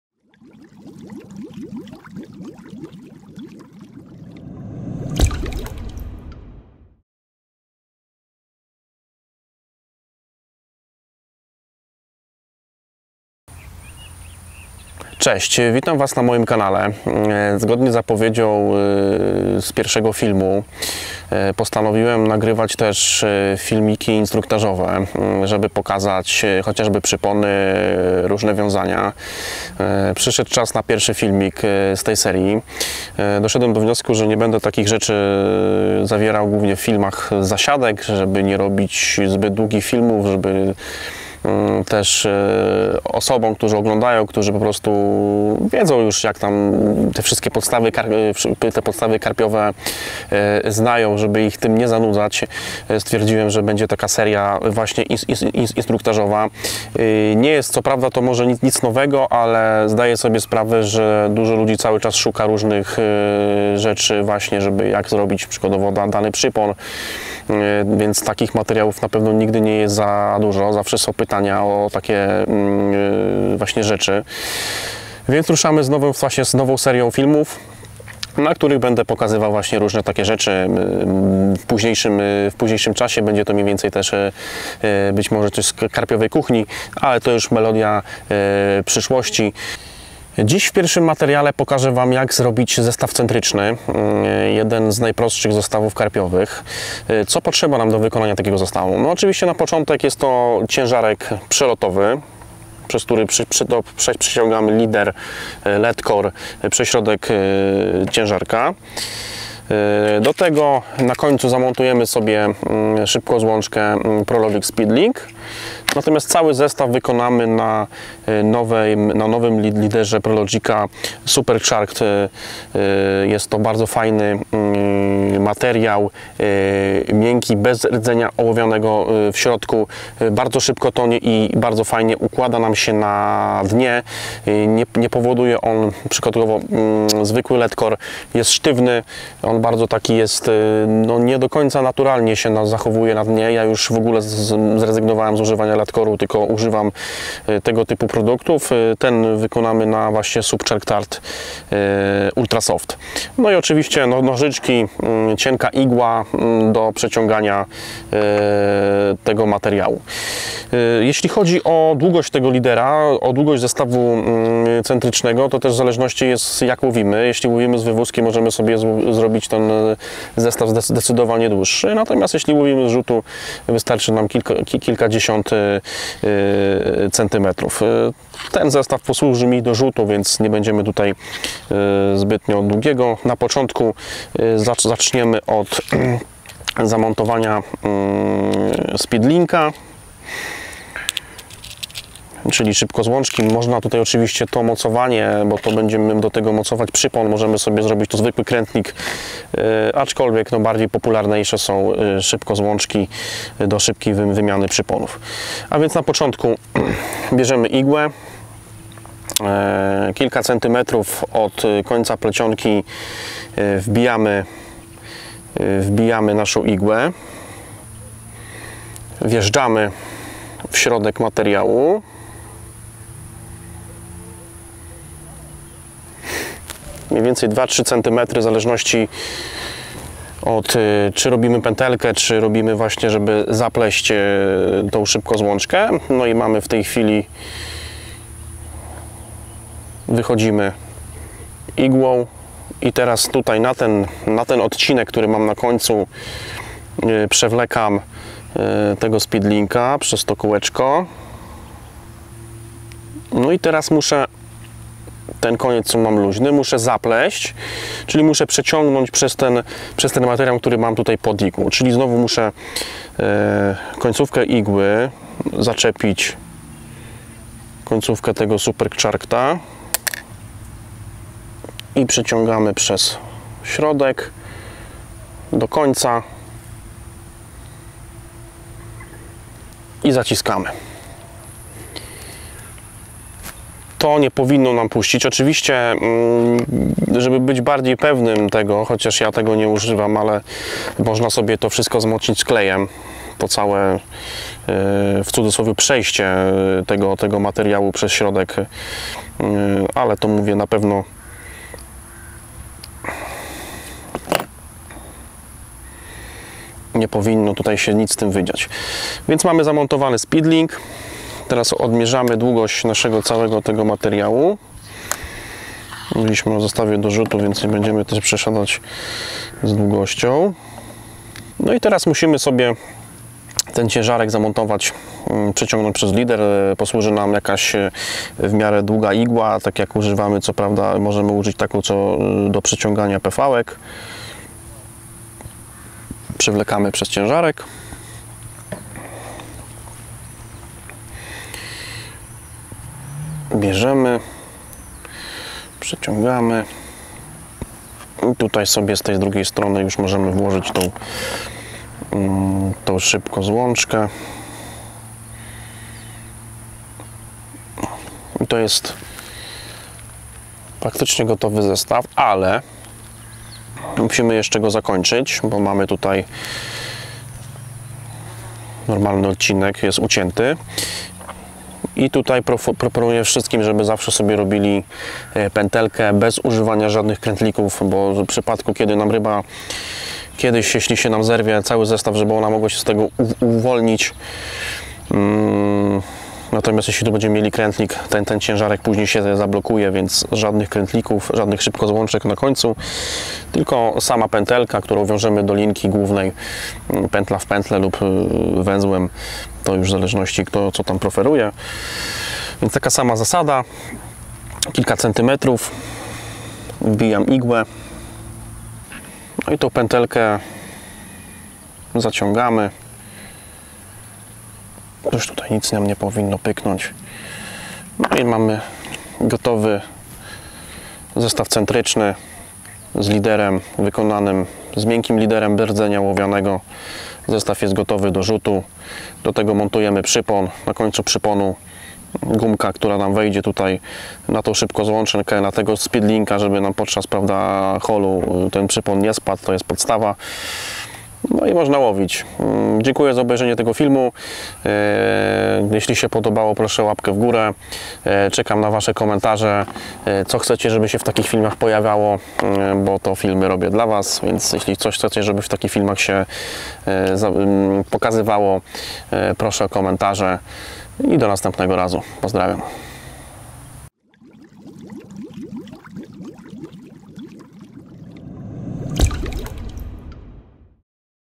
1# Zestaw karpiowy z ciężarkiem centrycznym. Krok po kroku
"Krok po Kroku" to nowa seria filmów instruktażowych, w której będą poruszane podstawowe zagadnienia wędkarstwa karpiowego. W pierwszym filmie zobaczycie jak wykonać zestaw z ciężarkiem centrycznym.